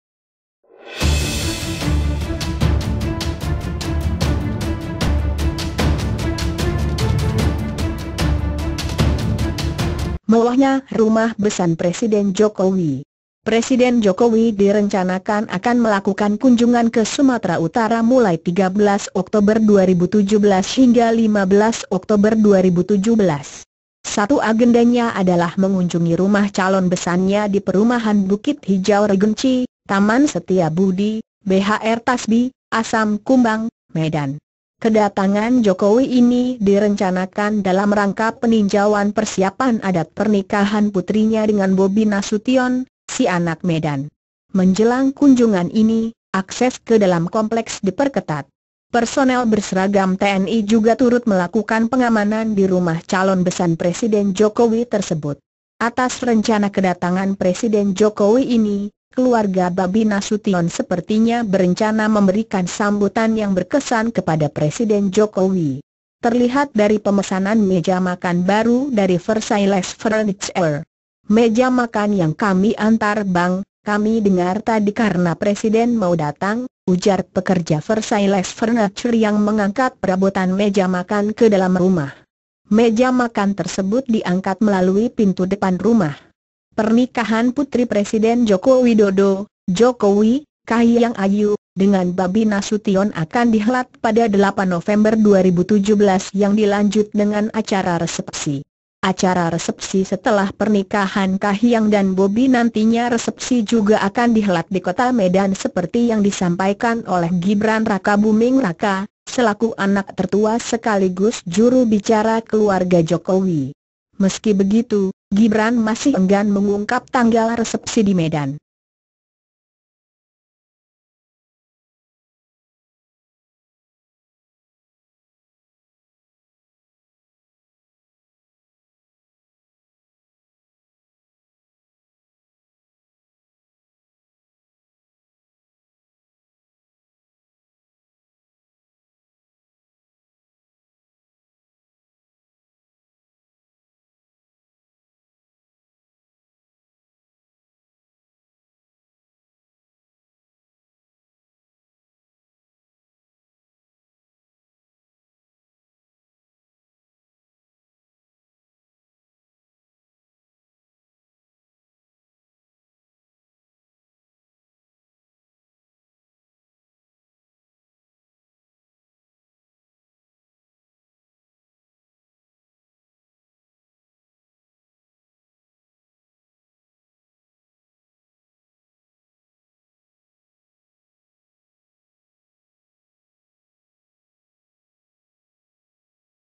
Mewahnya Rumah Besan Presiden Jokowi
Presiden Jokowi direncanakan akan melakukan kunjungan ke Sumatera Utara mulai 13 Oktober 2017 hingga 15 Oktober 2017.
Satu agendanya adalah mengunjungi rumah calon besannya di Perumahan Bukit Hijau Regency, Taman Setia Budi (BHR Tasbi), Asam Kumbang, Medan.
Kedatangan Jokowi ini direncanakan dalam rangka peninjauan persiapan adat pernikahan putrinya dengan Bobi Nasution, si anak Medan.
Menjelang kunjungan ini, akses ke dalam kompleks diperketat.
Personel berseragam TNI juga turut melakukan pengamanan di rumah calon besan Presiden Jokowi tersebut.
Atas rencana kedatangan Presiden Jokowi ini, keluarga Bobby Nasution sepertinya berencana memberikan sambutan yang berkesan kepada Presiden Jokowi.
Terlihat dari pemesanan meja makan baru dari Versailles Furniture.
"Meja makan yang kami antar bang, kami dengar tadi karena presiden mau datang," ujar pekerja Versailes Furniture yang mengangkat perabotan meja makan ke dalam rumah.
Meja makan tersebut diangkat melalui pintu depan rumah.
Pernikahan putri Presiden Joko Widodo (Jokowi), Kahiyang Ayu, dengan Bobby Nasution akan dihelat pada 8 November 2017 yang dilanjut dengan acara resepsi.
Acara resepsi setelah pernikahan Kahiyang dan Bobi nantinya resepsi juga akan dihelat di Kota Medan seperti yang disampaikan oleh Gibran Rakabuming Raka, selaku anak tertua sekaligus juru bicara keluarga Jokowi.
Meski begitu, Gibran masih enggan mengungkap tanggal resepsi di Medan.